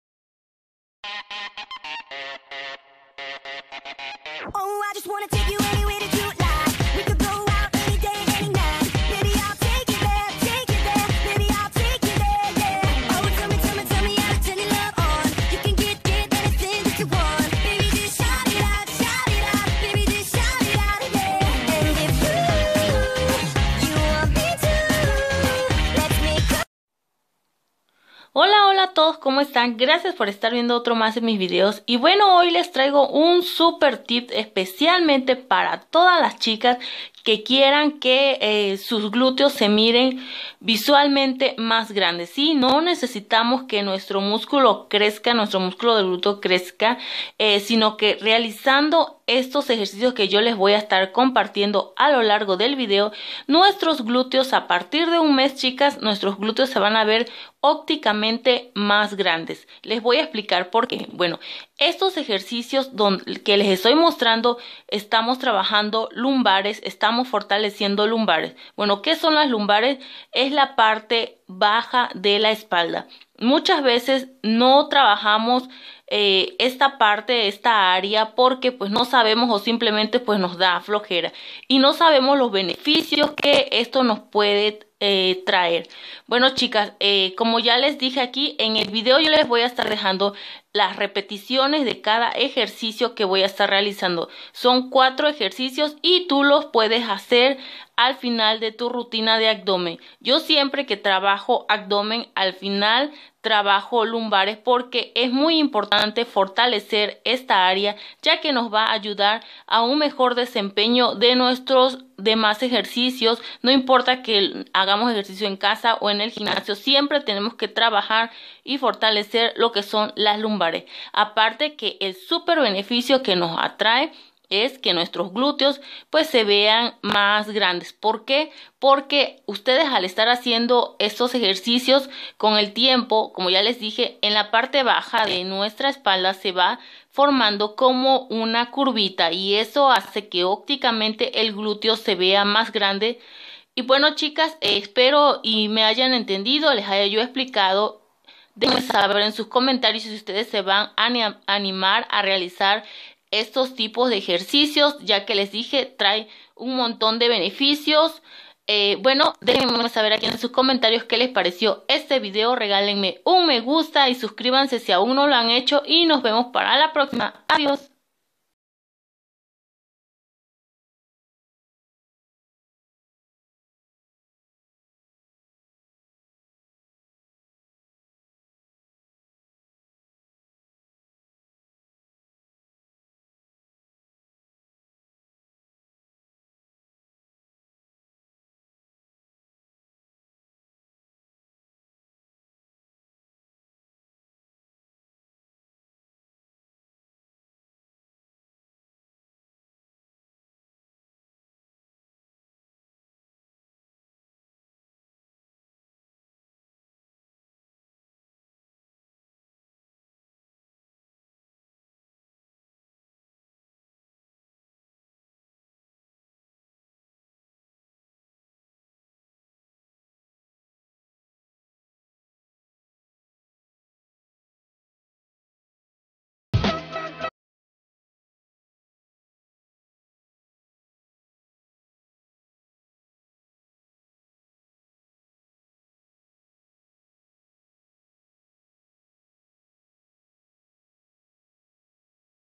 " SUPER TIP PARA TENER GLUTEOS MAS GRANDES"(VISUALMENTE)
PLATICA DE COMO CONSUMIR TUS ALIMENTOS PARA PERDER PESO Y AUMENTAR MASA MUSCULAR" LOS MACRONUTRIENTES PARA CALCULAR TU INGESTA CALORICA PARA PERDER PESO O AUMENTAR MASA MUSCULAR. MUY FACIL PARA CALCULAR TUS MACRONUTRIENTES( para perder peso)..